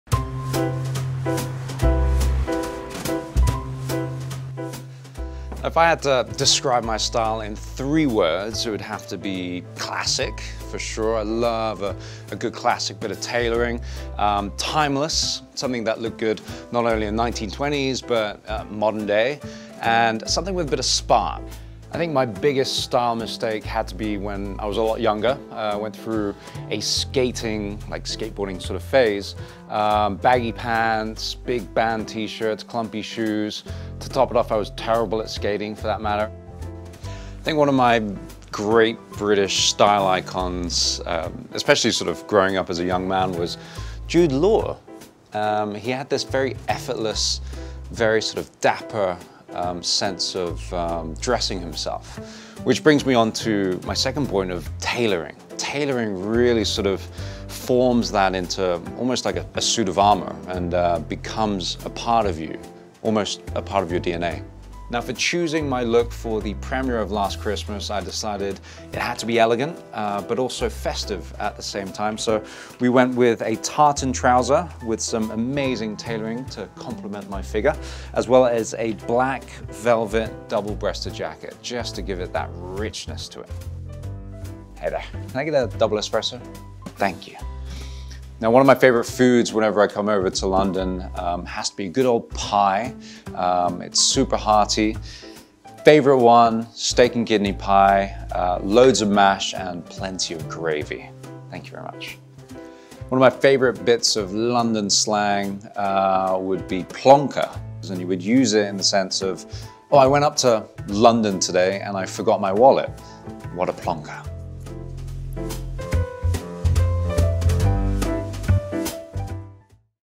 RALPH LAUREN | Henry Golding’s London Style
Before Henry Golding hit the red carpet for the London premiere of his latest film, we asked the "Last Christmas" star to share his take on style, British slang, and the confidence that comes with a well-tailored suit.